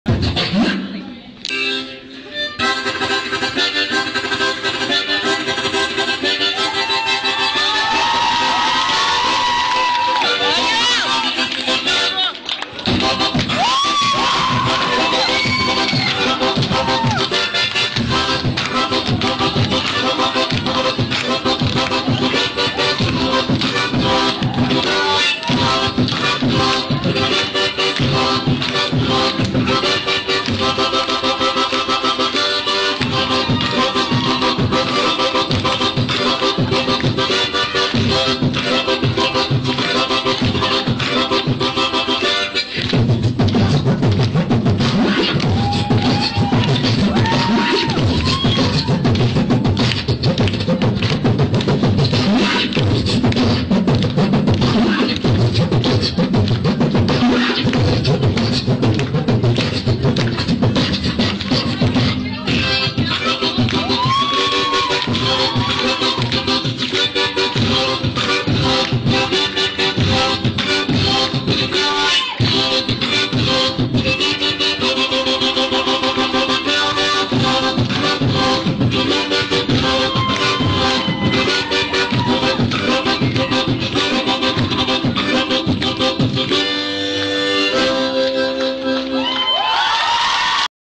РАИЛЬ АРСЛАНОВ - Школьное выступление 12 лет назад | БИТБОКС + ГУБНАЯ ГАРМОШКА
29 октября 2010 год.